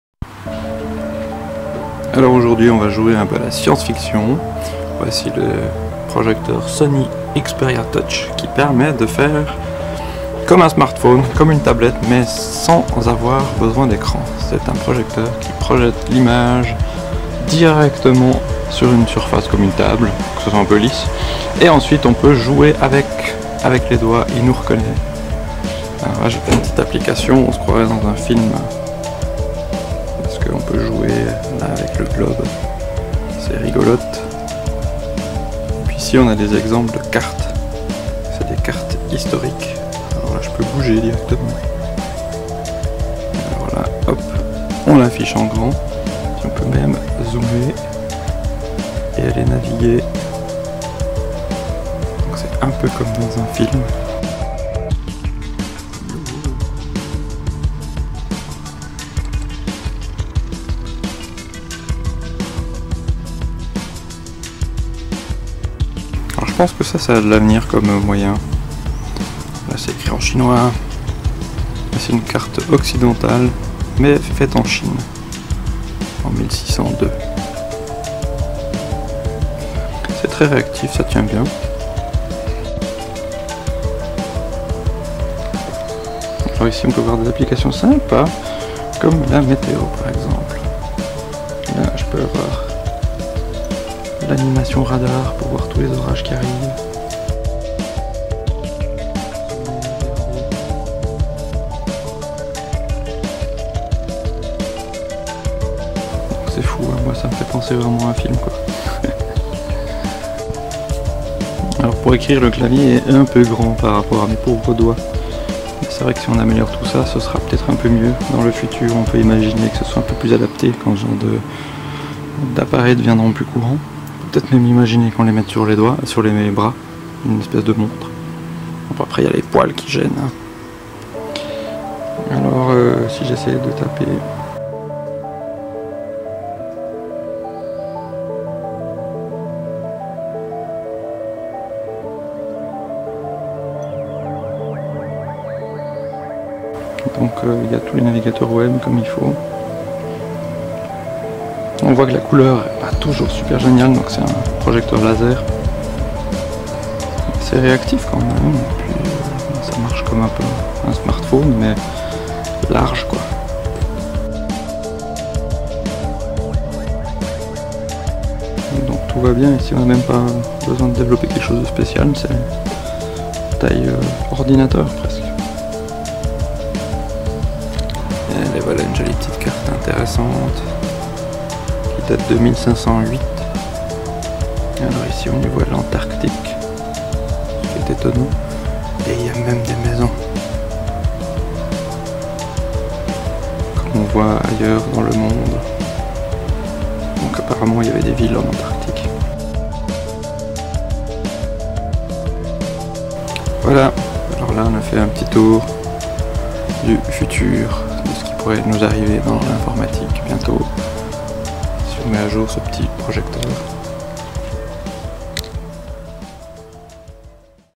Demo projecteur Sony Xperia Touch - Transformer une table en écran tactile
Voici une démo du projecteur Sony Xperia Touch.
C'est un projecteur tactile !
C'est une "tablette" android sans écran. Mais qui permet de transformer n'importe quelle surface (plane) en surface tactile.

Je pense que ça a de l'avenir comme principe.

Mon génial micro-enregistreur numérique